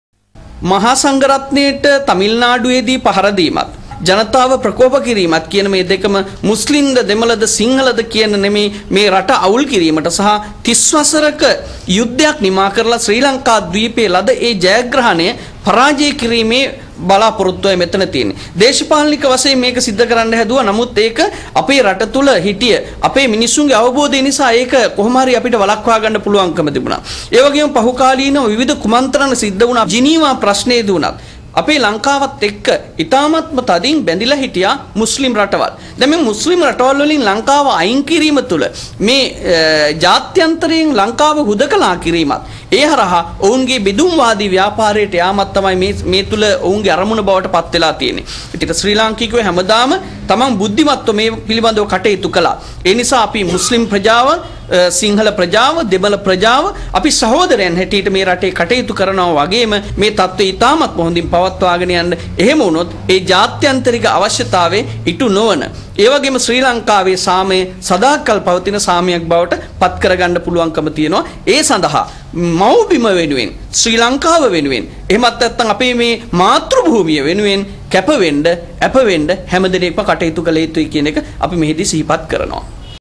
Response of Buddhist monks regarding the attacks against Muslims (with Tamil Subtitles) - Part 2
Dr. Rev Madagoda Abayathissa Thero's response regarding recent ethnic clashes between Sinhalese and Muslims